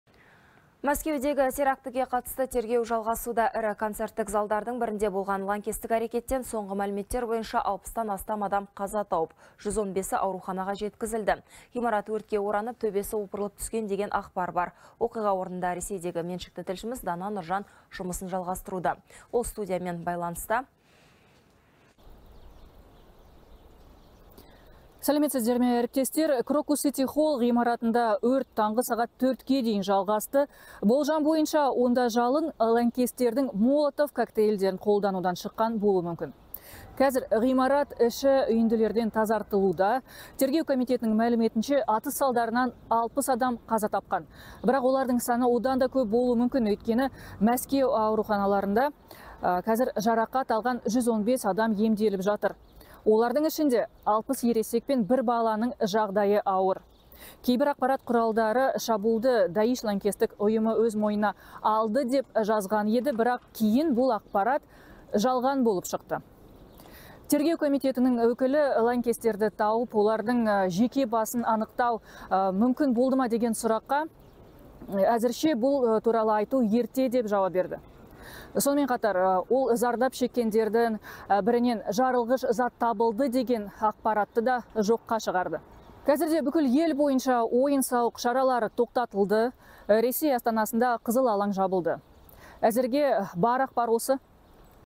Мәскеудегі теракт құрбандарының саны артуы мүмкін
Мәскеудегі терактіге қатысты тергеу жалғасуда.